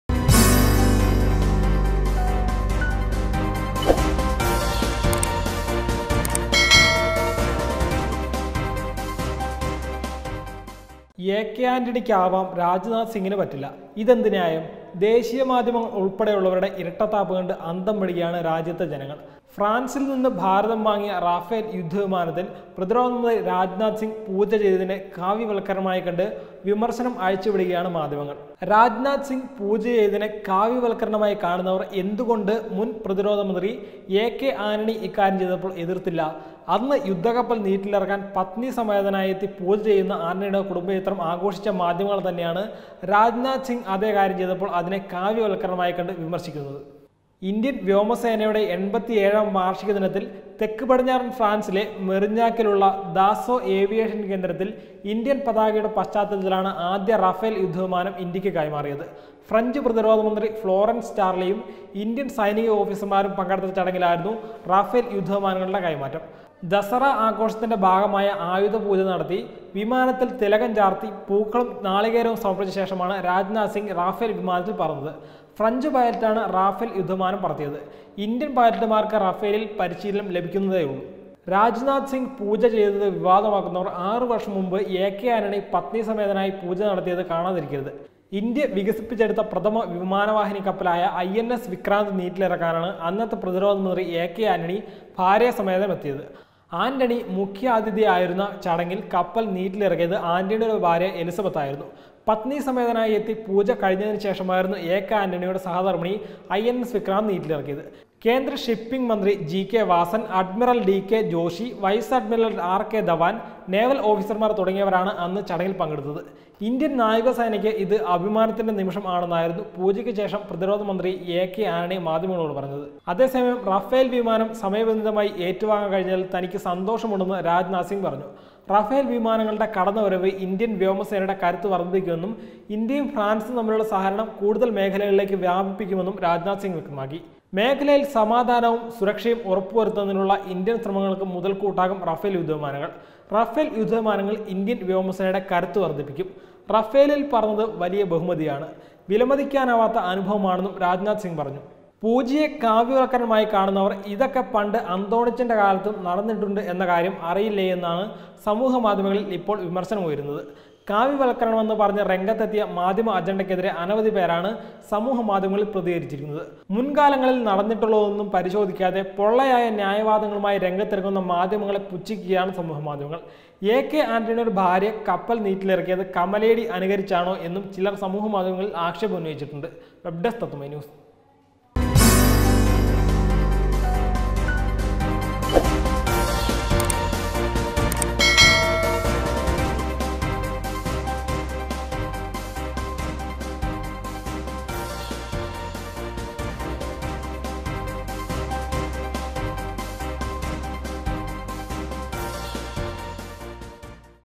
എ.കെ ആന്‍റണിക്ക് ആവാം; രാജ് നാഥ് സിംഗിന് പറ്റില്ല !
എ കെ ആന്റണിക്ക് ആവാം.. രാജ്നാഥ് സിംഗിന് പറ്റില്ല ! ഇതെന്ത് ന്യായം.  ദേശീയ മാധ്യമങ്ങളുള്‍പ്പെടെയുള്ളവരുടെ ഇരട്ടത്താപ്പ് കണ്ട് അന്തം വിടുകയാണ് രാജ്യത്തെ ജനങ്ങള്‍. ഫ്രാൻസിൽ നിന്നും ഭാരതം വാങ്ങിയ റാഫേൽ യുദ്ധവിമാനത്തിൽ  പ്രതിരോധമന്ത്രി രാജ്നാഥ് സിംഗ് പൂജചെയ്തതിനെ 'കാവിവൽക്കരണ'മായി കണ്ട് വിമര്‍ശനം അഴിച്ചുവിടുകയാണ് മാധ്യമങ്ങള്‍.